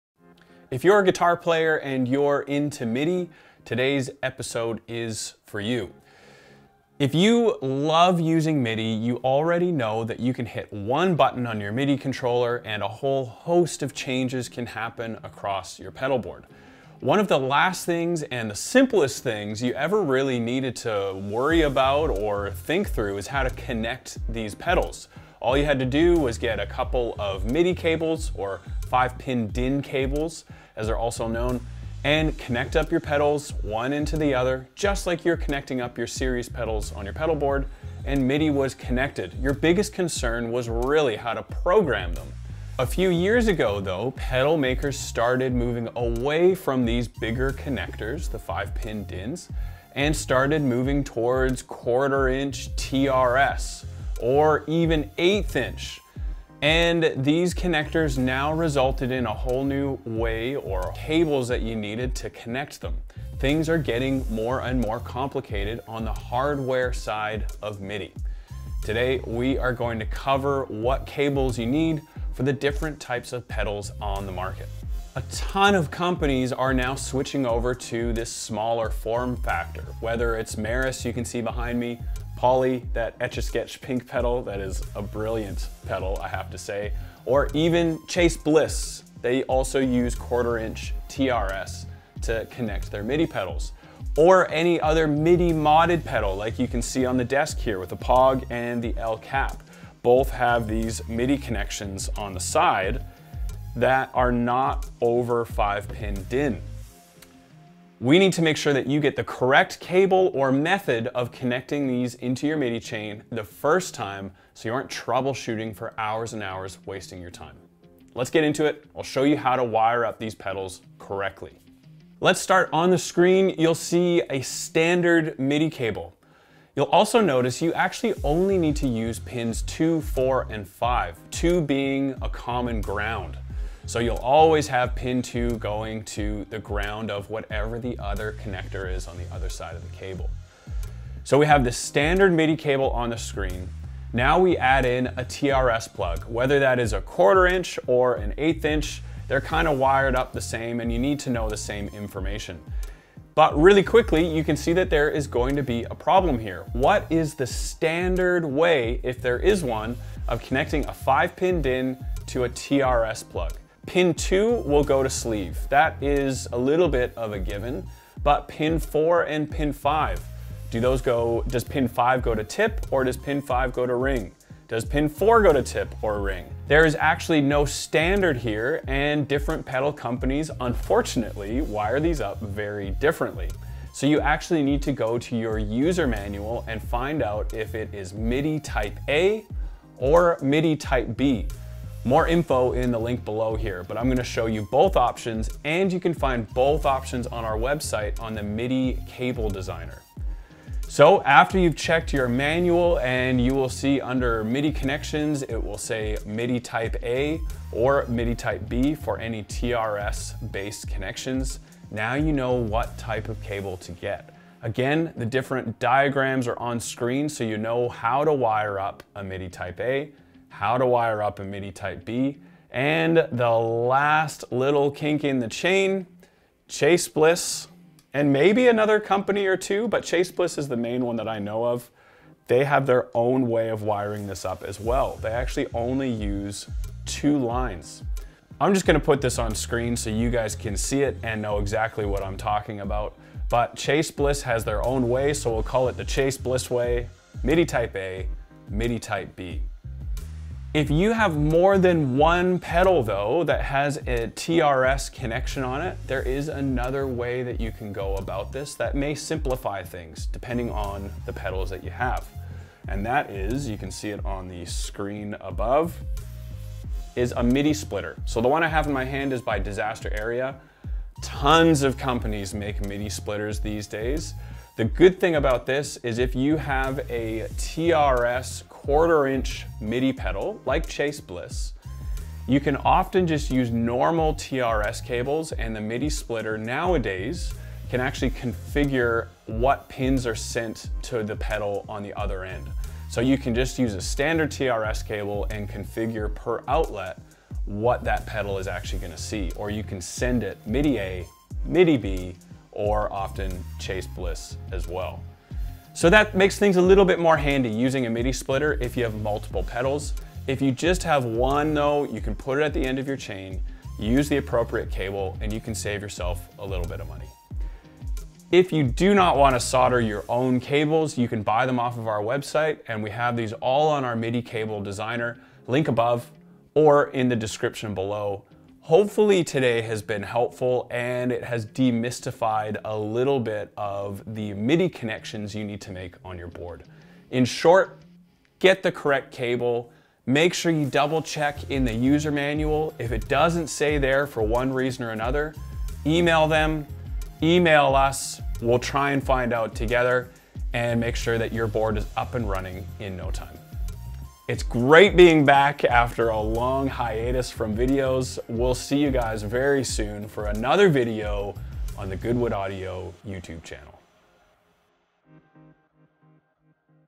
Connect Your TRS Midi Pedals Right The First Time!
In the last few years, pedal builders have started moving away from the standard 5 Pin DIN plug for their mini connections.  In the name of space saving, pedal builders have moved towards 1/4" and 1/8" TRS plugs instead.

This is great - except when you need to figure out how to connect them.
You now need to know what midi configuration the pedal builder has chosen for their pedal:
1. Midi Type A
2. Midi Type B
3. Chase Bliss (take a stab at which company uses this one!)

In this video Grant goes through what the difference is, how to figure it out for your pedal and a couple other options to keep connections simple and predictable so you can get back to playing guitar as quickly as possible!